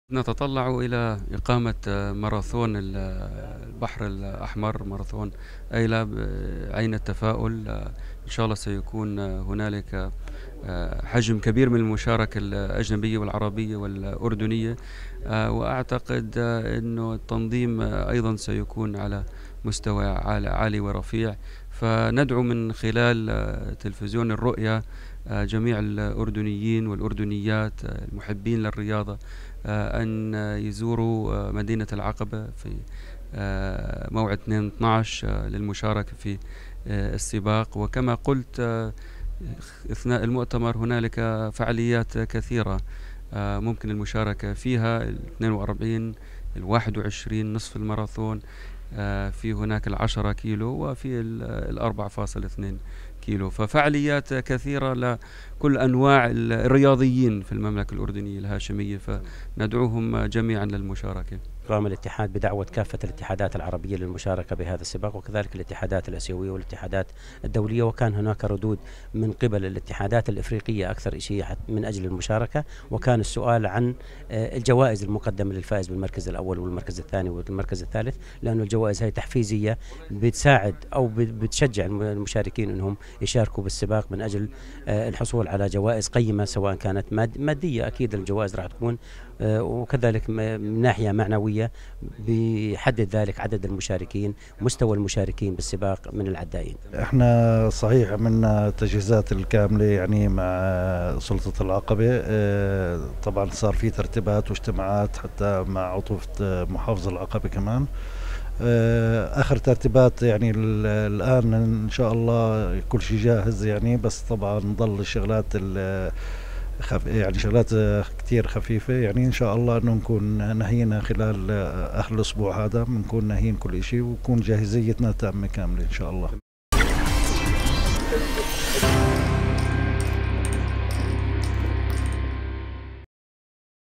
مؤتمر تحضيرات ماراثون أيله البحر الأحمر 2011
المؤتمر الصحفي  الذي عقد يوم امس الاحد  للحديث عن ماراثون ايله البحر الاحمر الذي سينطلق يوم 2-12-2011 في مدينة العقبة